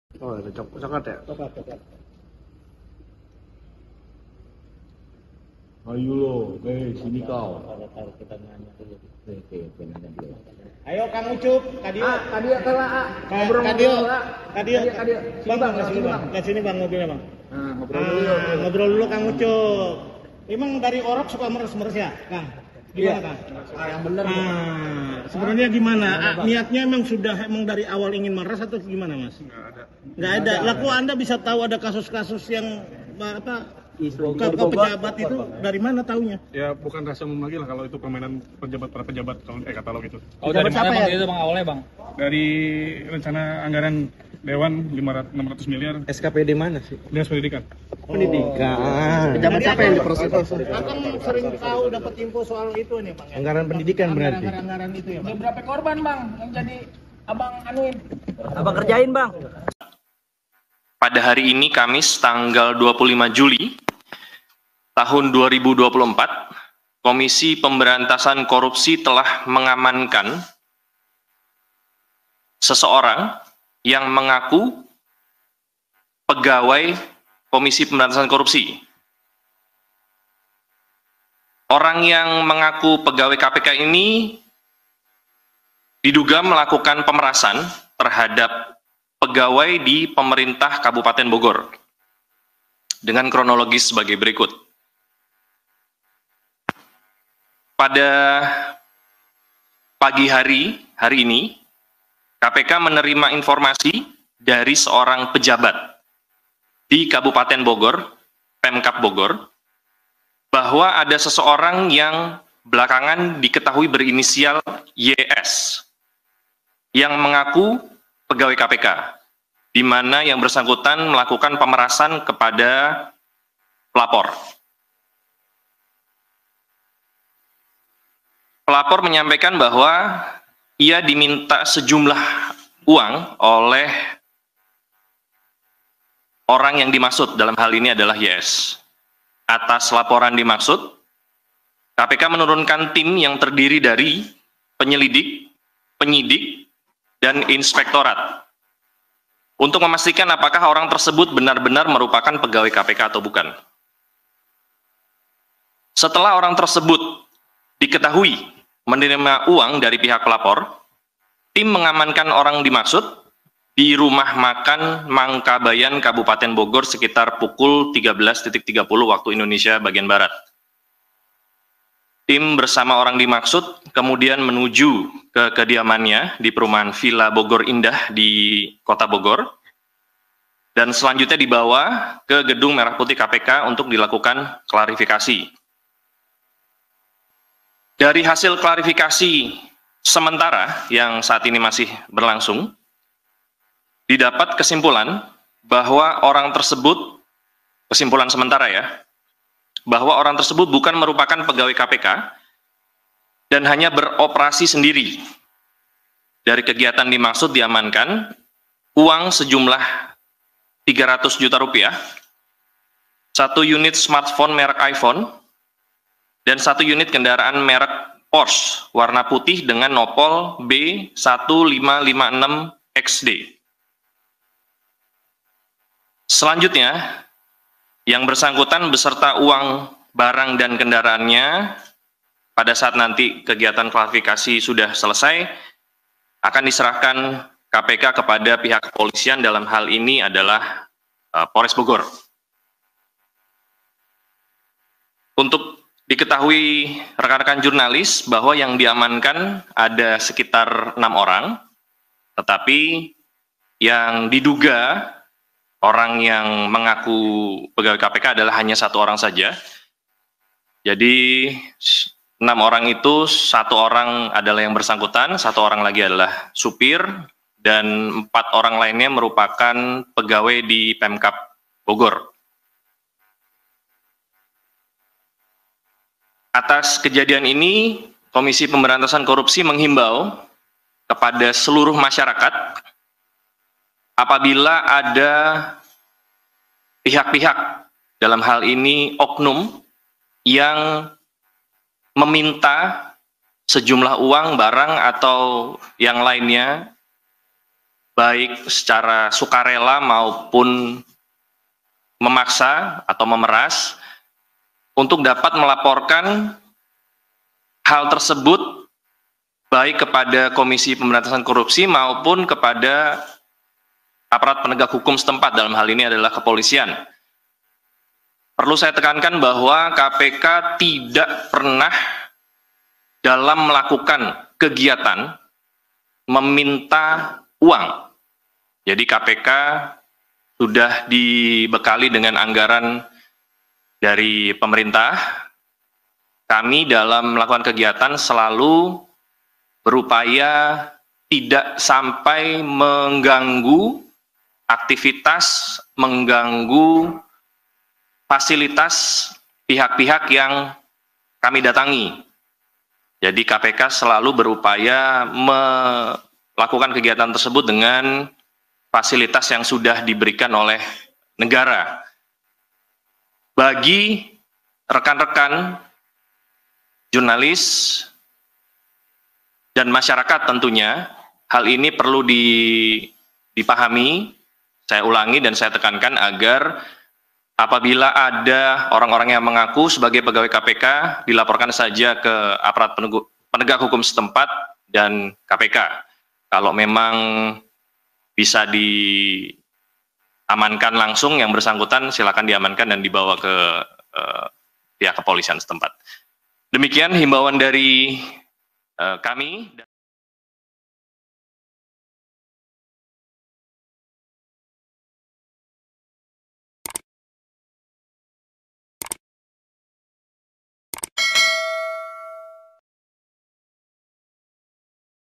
Pegawai Gadungan KPK yang Diduga Peras Pejabat Rp 300 Juta Digiring ke Mapolres Bogor
Pria yang mengaku bekerja di Komisi Pemberantasan Korupsi (KPK) Yusup Sulaeman dibawa ke Mapolres Bogor, Jawa Barat, Jumat (26/7/2024) dini hari.
Pantauan Kompas.com, Yusuf dibawa turun dari lantai 2 Gedung Merah Putih KPK pada pukul 00.05 WIB.
Dengan tangan diborgol, pegawai gadungan KPK itu langsung digiring polisi untuk masuk mobil Innova berpelat polisi yang telah menunggu di lobi kantor Komisi Antirasuah itu.
Adapun KPK menangkap Yusup yang diduga memeras pejabat di lingkungan Pemerintah Kabupaten (Pemkab) Bogor, Kamis (25/7/2024) siang.
Dalam konferensi pers, Kamis sore, Juru Bicara KPK Tessa Mahardhika Sugiarto menyampaikan, pria bertubuh gempal itu mengaku sebagai pegawai KPK.
Awalnya, kata Tessa, KPK menerima informasi dari salah satu pejabat di lingkungan Pemkab Bogor bahwa Yusuf yang mengaku pegawai KPK diduga melakukan pemerasan. Yusuf disebut meminta sejumlah uang Rp 300 juta kepada pejabat Pemkab Bogor tersebut.
Setelah menciduk Yusuf, Tim KPK kemudian membawa terduga ke rumahnya di kawasan Villa Bogor Indah, Kota Bogor. Setelah itu, Yusuf dibawa ke Gedung Merah Putih KPK untuk menjalani klarifikasi.
Dari operasi senyap itu, KPK menyita uang Rp 300 juta, satu ponsel merek iPhone, dan satu unit mobil Porsche warna putih dengan no pol B 1556 XD.
Simak selengkapnya dalam tayangan berikut ini.
Penulis: Irfan Kamil
Video Jurnalis: Irfan Kamil
Penulis Naskah: Xena Olivia
Produser: Nursita Sari
Video Editor: Xena Olivia